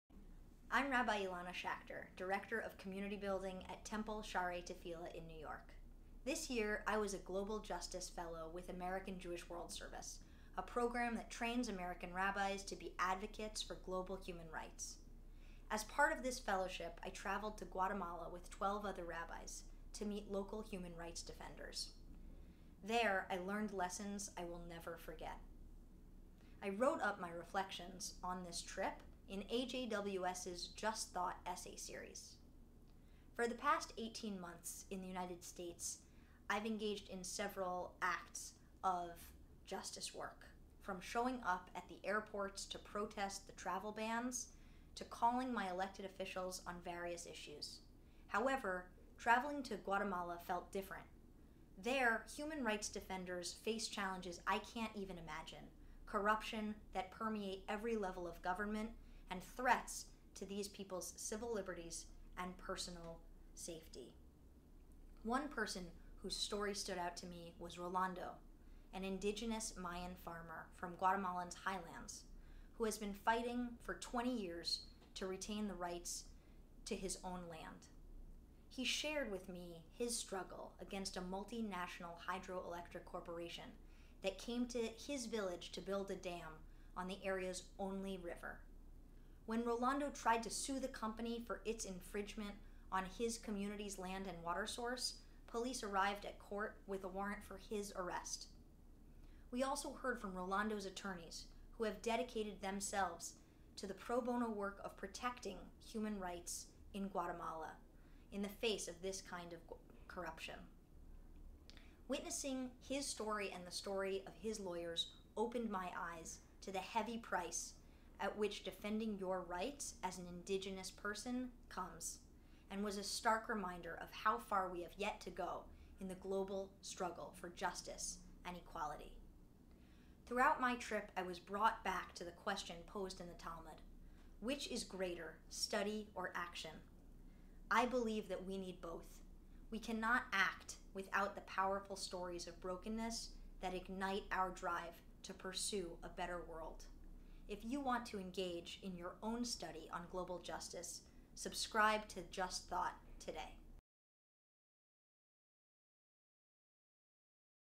Rabbi Ilana Schachter on Her Trip to Guatemala with AJWS
Rabbi Ilana Schachter, an AJWS Global Justice Fellow and Director of Community Building at Temple Shaaray Tefila in New York, shares her experience traveling to Guatemala with AJWS her writing about it for our JustThought series.